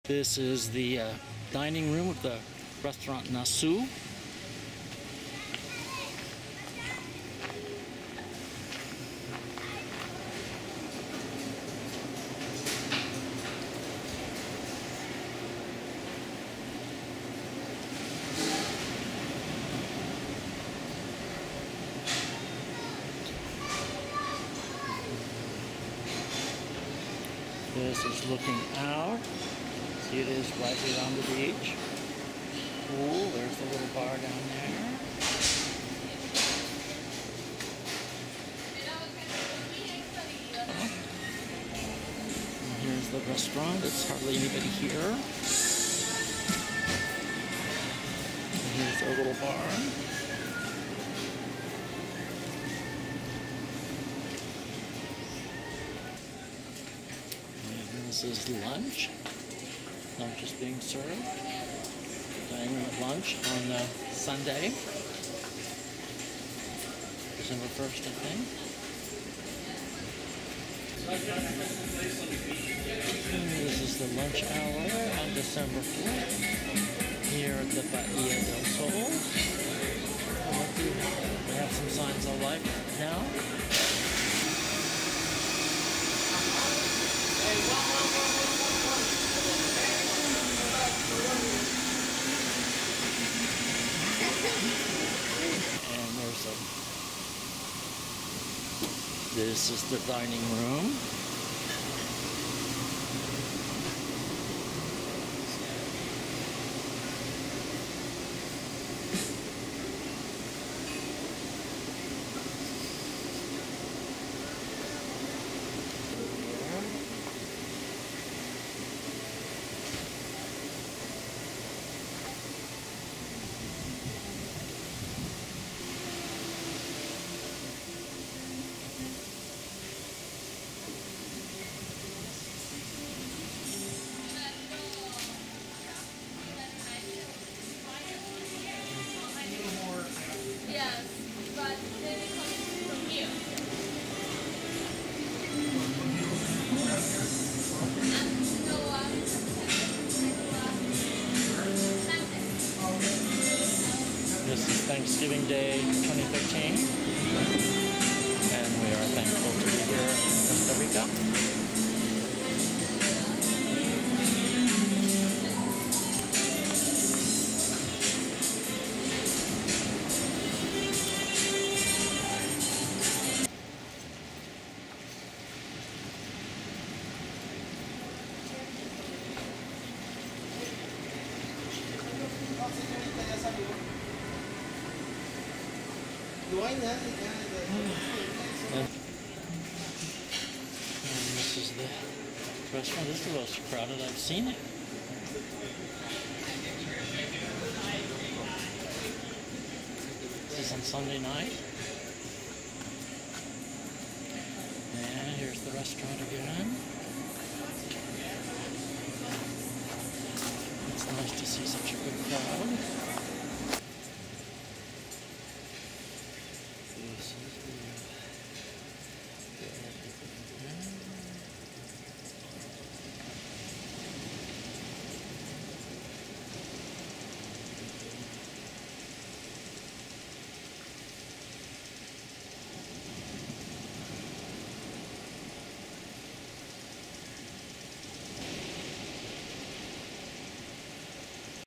NASU restaurant, Bahía del Sol Beachfront Hotel, Playa Potrero, Costa Rica
This shows the in-house restaurant, the restaurant NASU, at the Bahía del Sol Beachfront Hotel in Playa Potrero, Costa Rica. This video shows the restaurant on several different days and at different times of the day.  The breakfast here was very good. Other meals were uneven--some were good and others not so-good. Prices were pretty high, but no higher than average in Playa Potrero. Overall, the level of the cuisine in Playa Potrero was nowhere close to that of the Mexican resorts we have been to.  These views span late November to early December of 2014. As can be seen from the final shots here, there were a lot more people on December 4 than when we arrived a week earlier.